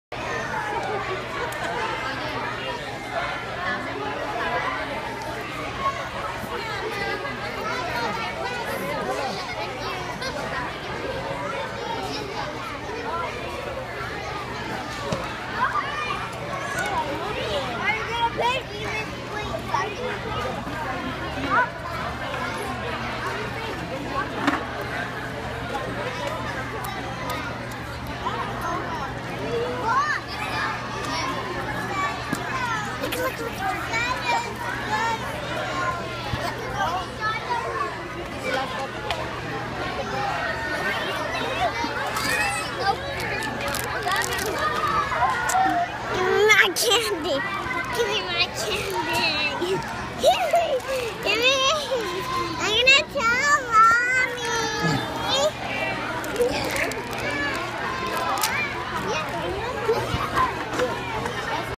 Halloween Cravings
Olivia's candy starts to look good.